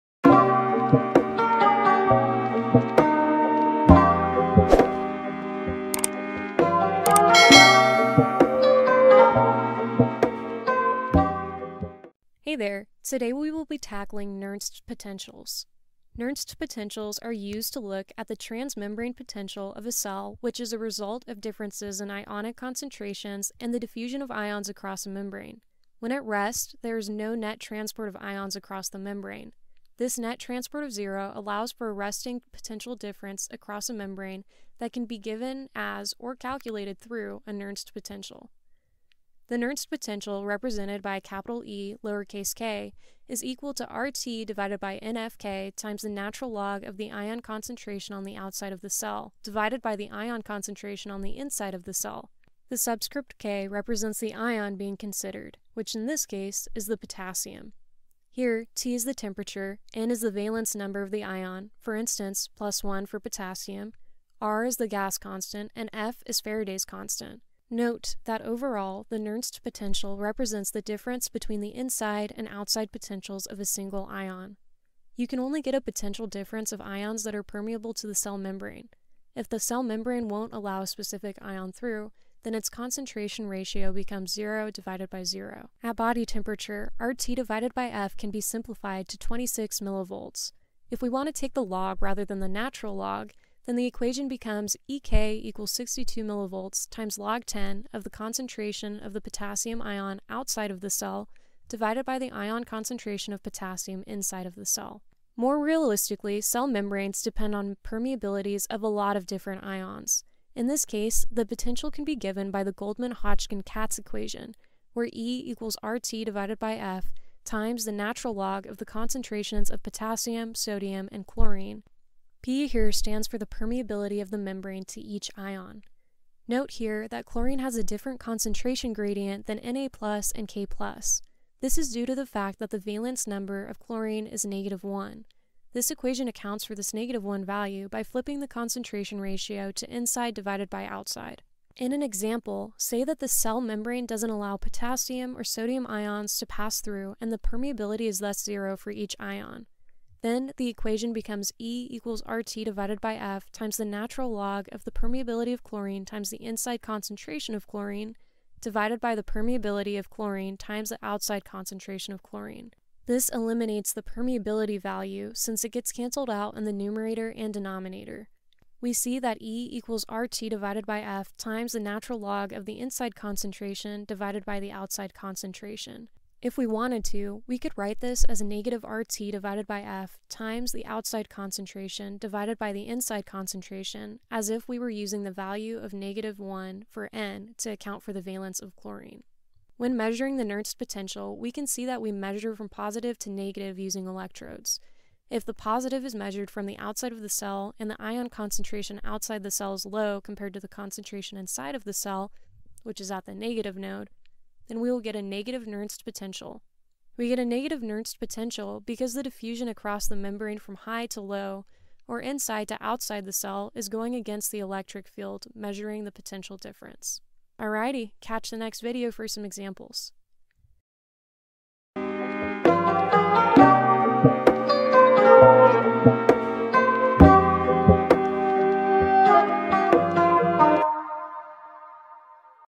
The Nernst Potential and The Cell Membrane | Bioinstrumentation Design
BENG 186B: Principles of Bioinstrumentation Design (video 20)

Nernst potentials are used to look at the transmembrane potential of a cell, which is a result of differences in ionic concentrations and the diffusion of ions across a membrane. When at rest, there is no net transport of ions across the membrane. This net transport of 0 allows for a resting potential difference across the membrane that can be given as or calculated through a Nernst potential.

Want to give us some feedback so that we can better help you? Let us know what you think about the video here: